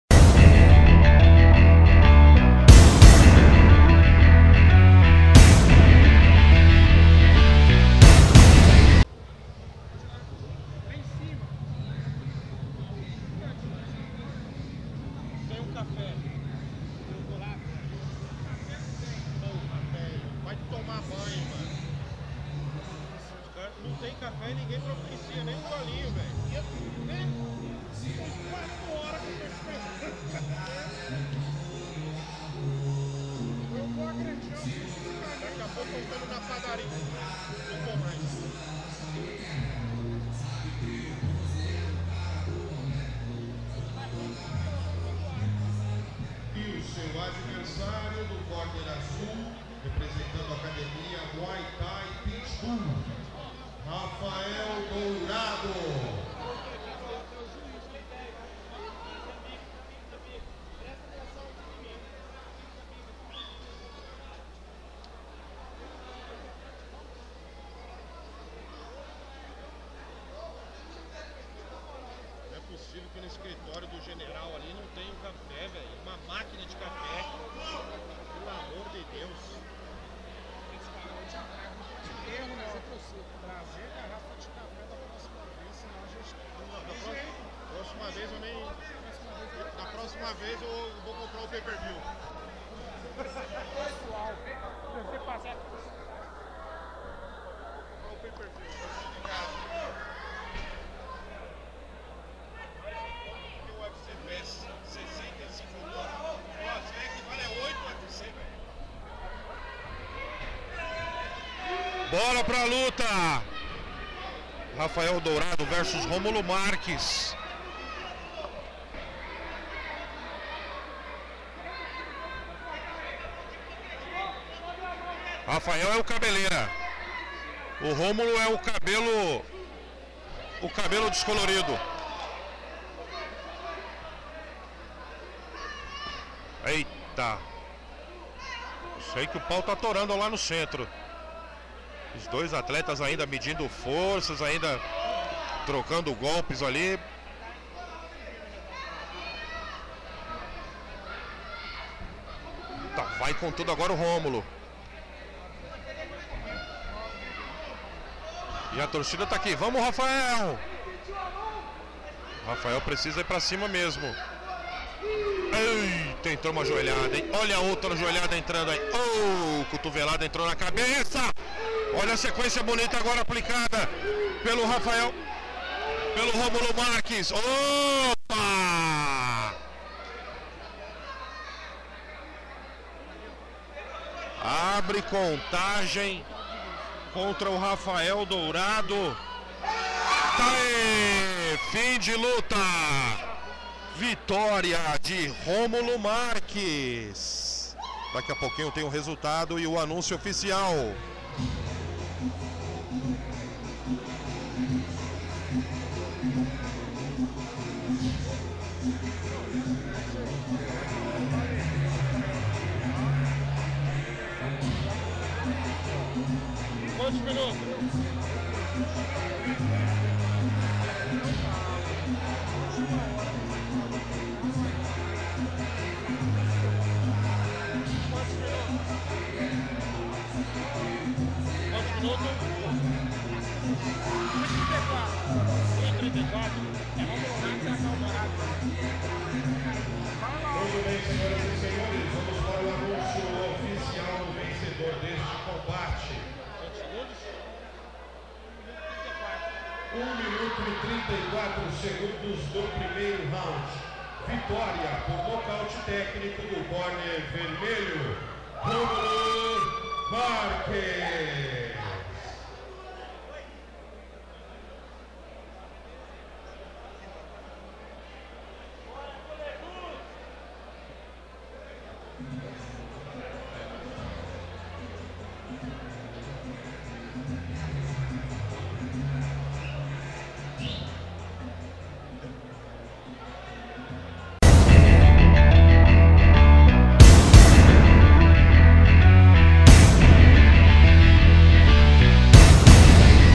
Romulo vs Rafael - X1 Kombat 10
Em combate de MuayThai amador até 70kg, Romulo Marques (Team Leeh) venceu Rafael Dourado (MuayThai Pitt Bull) por nocaute téncico no primeiro round.